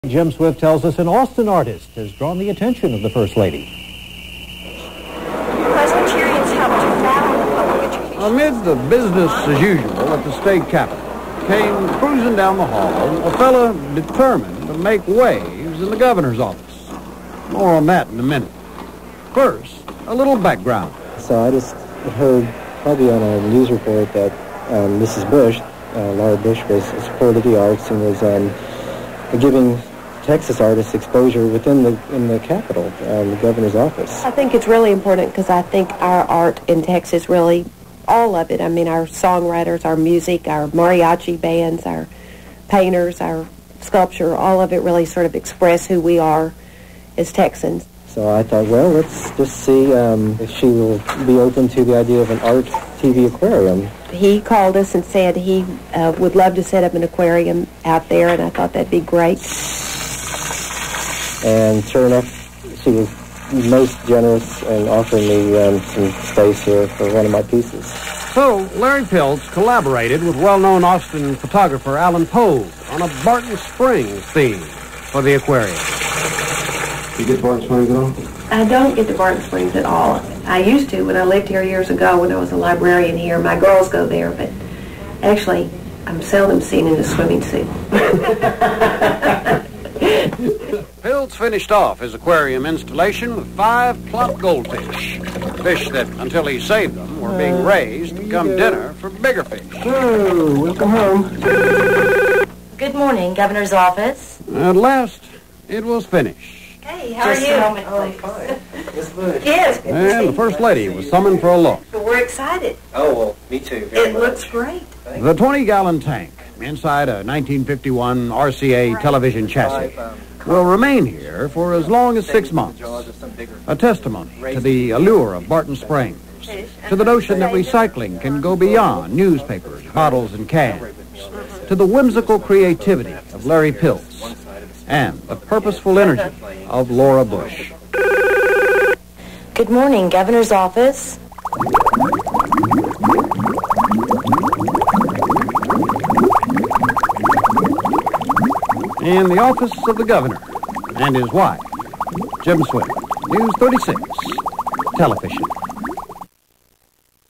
Laura Bush welcoming Artist Larry Piltz to the State Capitol
Beyond being a case of near-opposite political bedfellows, this is documentary footage of Future First Lady Laura Bush welcoming Austin Artist Larry Aaron Piltz to the Governor's Office in the Texas State Capitol in 1997.

Piltz exhibited two of his celebrated Mixed Media Art Sculpture Aquariums in Bush's Office for a total of three years, including the crucial years of Bush's victorious 2000 Presidential Election Campaign and Inauguration Day 2001.

Documentation of authenticity will be provided.  Delivery and installation by the Artist can be arranged.

(Video is from KXAN Channel 36 TV, Austin, Texas; Jim Swift, Producer)